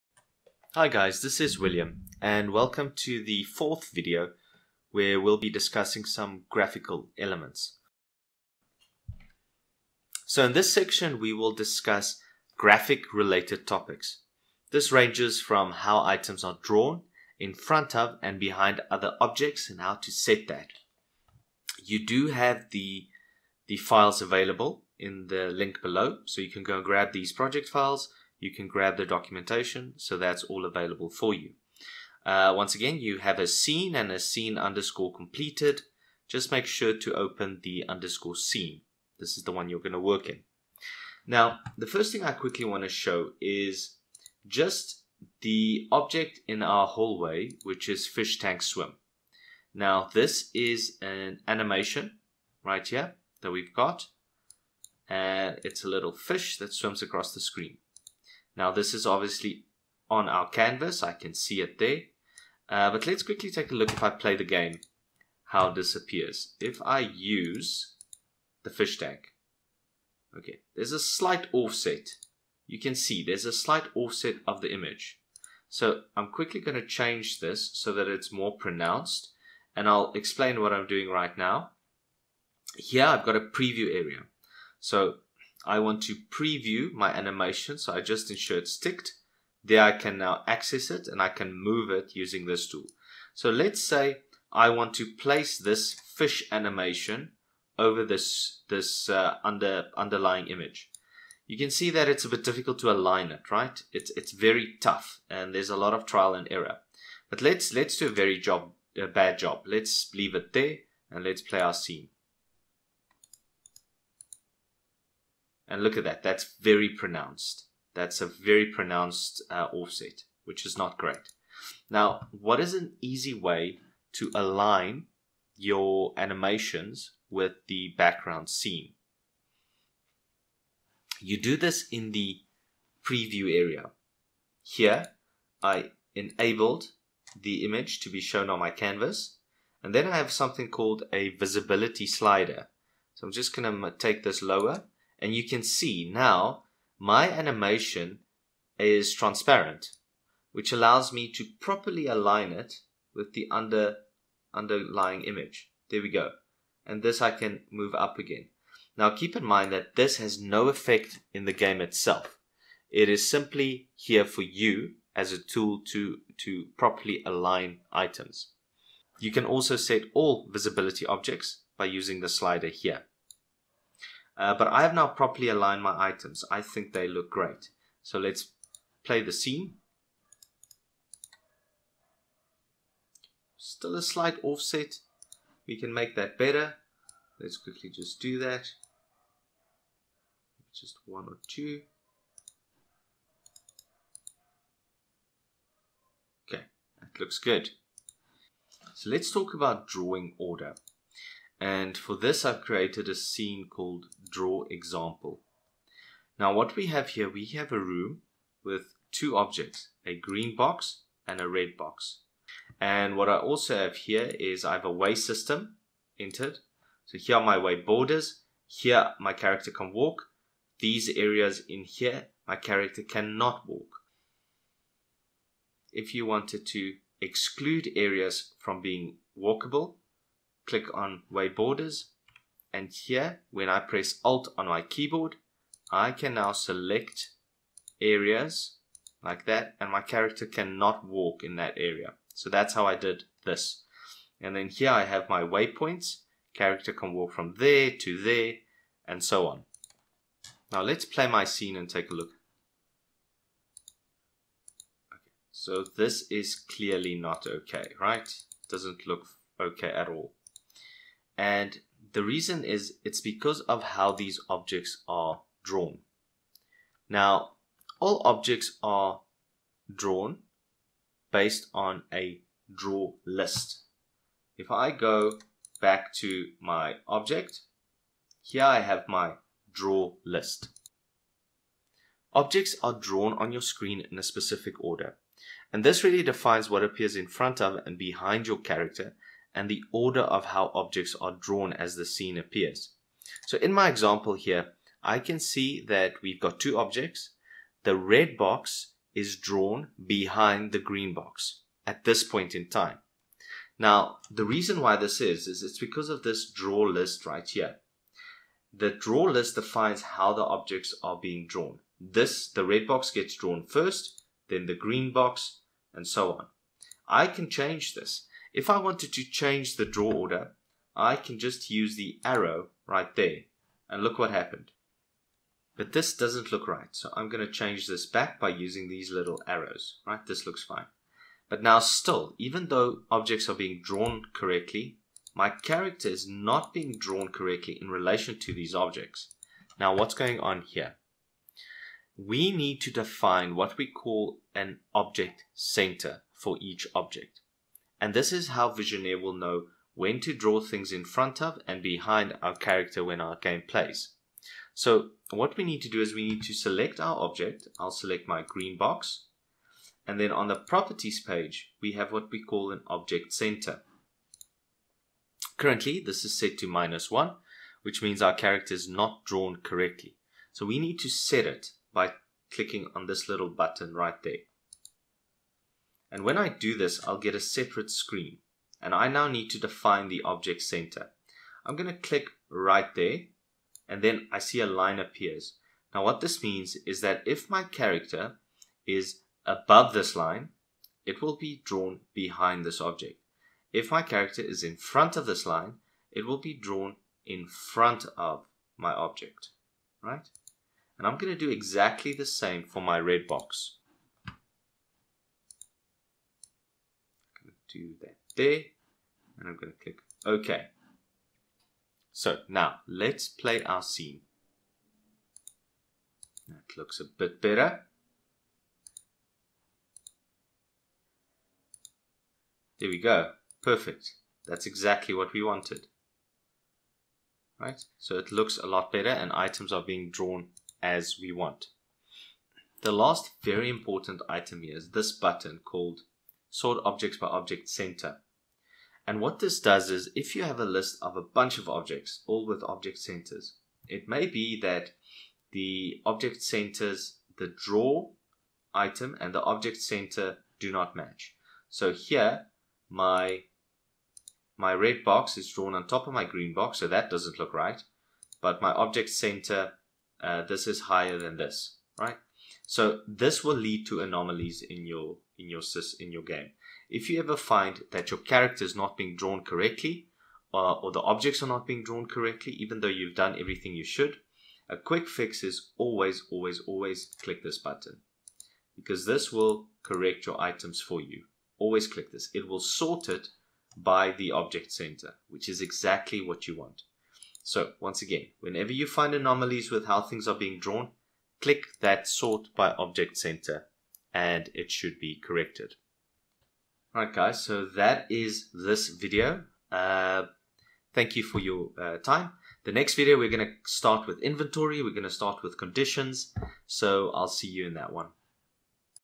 Visionaire Studio Basics - Video 4-1 - Graphical Elements
In this video we'll be looking at dealing with graphical elements.